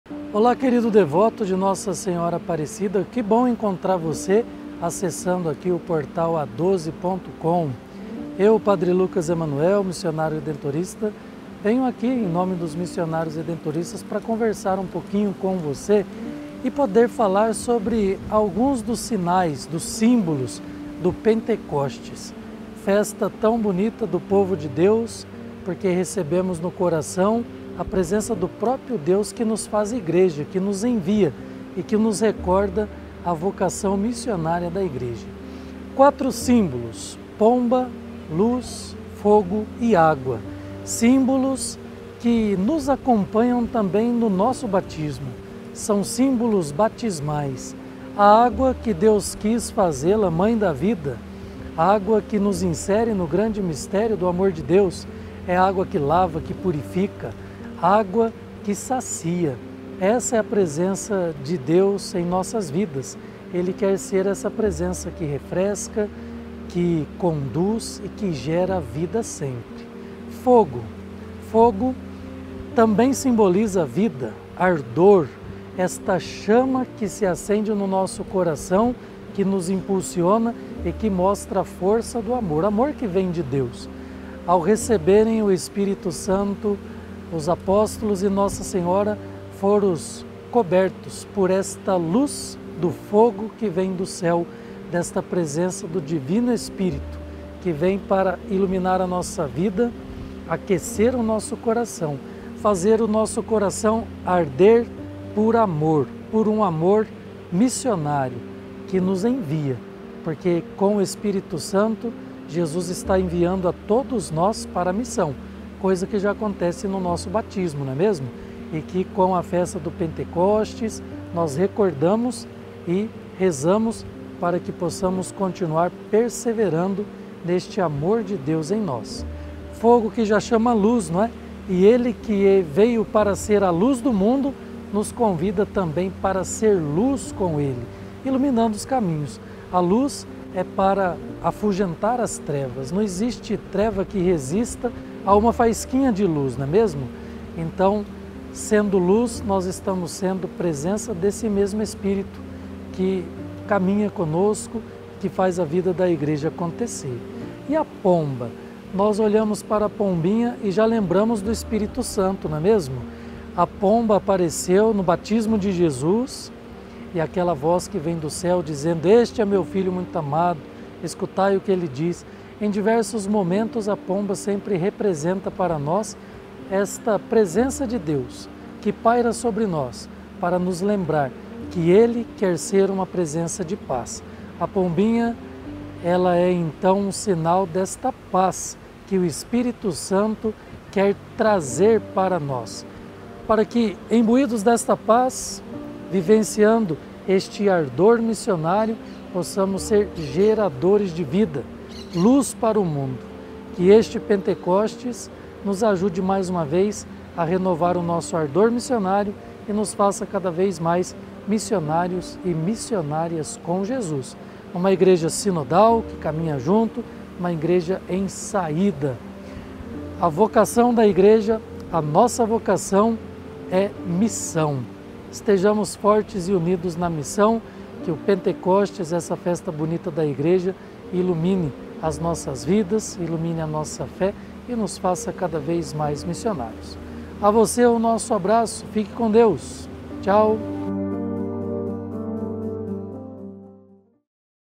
Símbolos de Pentecostes: Espírito Santo
Nesse tempo de Pentecostes, Pe. Lucas Emanuel, C.Ss.R. explica aos fiéis alguns dos símbolos desta importante Solenidade para nós, católicos. Entendemos melhor o significado do fogo, da luz, da água e da pomba.